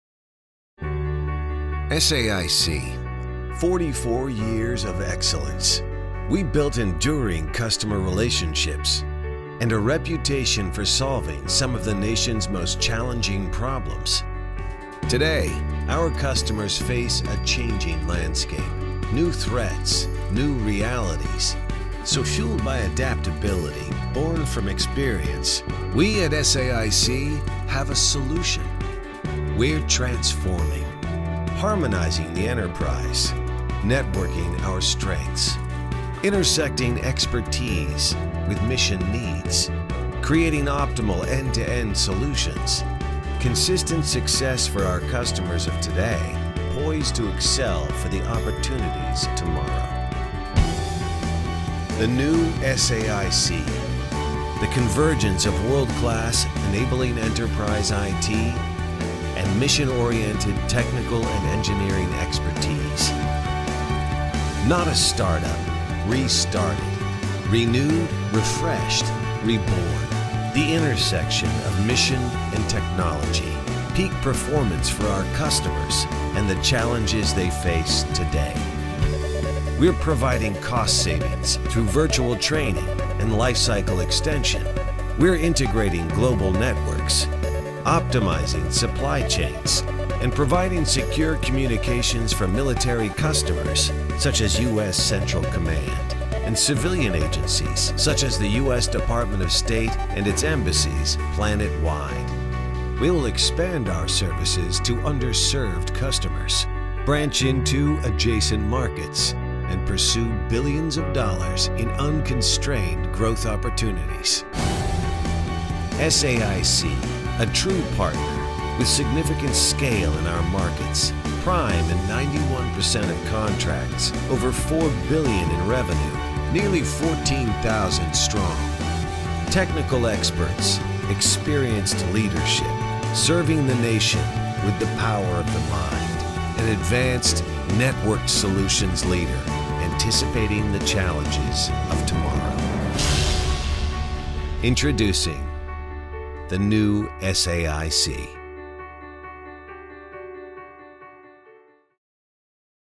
The Next Generation of SAIC
The next generation of Science Applications International Corporation (SAIC) is a leading technology integrator providing full lifecycle services and solutions in the technical, engineering, and enterprise IT markets.